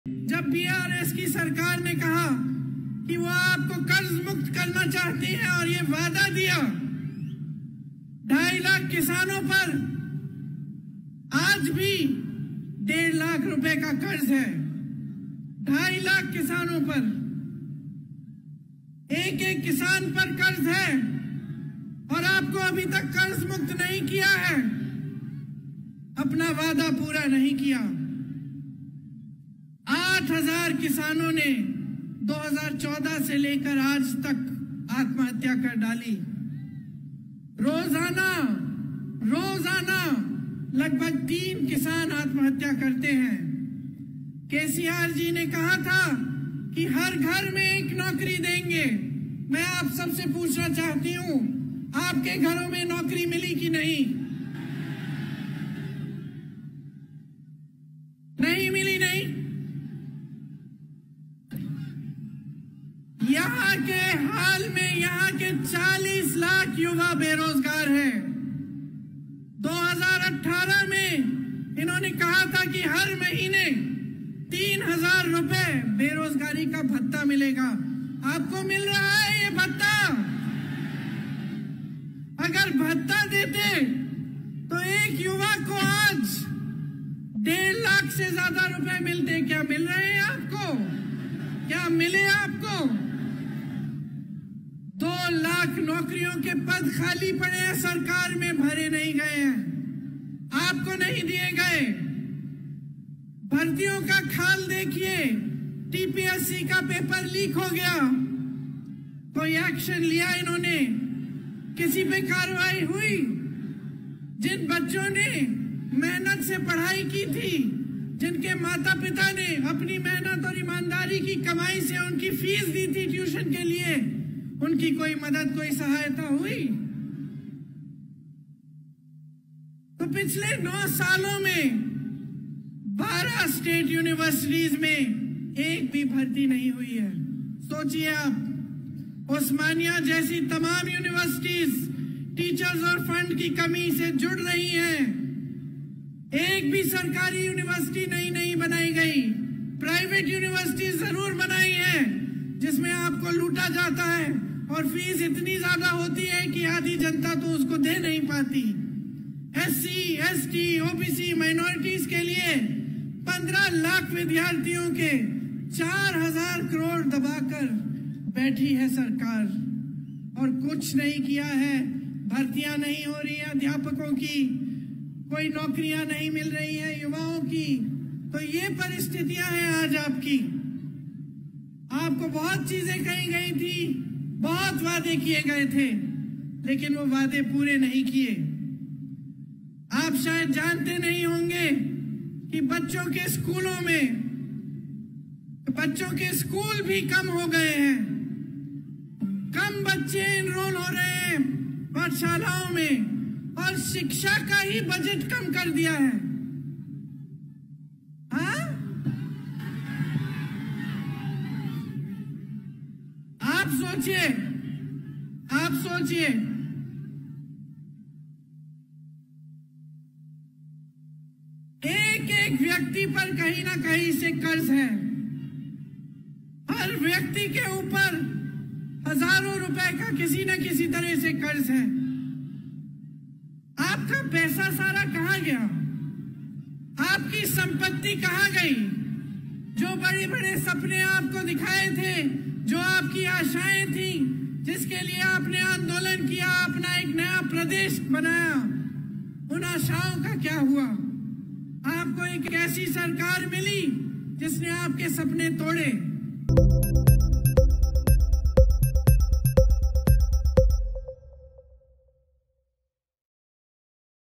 రైతులకు రుణమాఫీ చేస్తామన్న హామీని ప్రభుత్వం నిలబెట్టుకోలేదు | T Congress | Saroor Nagar | Mango News
రైతులకు రుణమాఫీ చేస్తామన్న హామీని ప్రభుత్వం నిలబెట్టుకోలేదు. Yuva Sangharshana Sabha.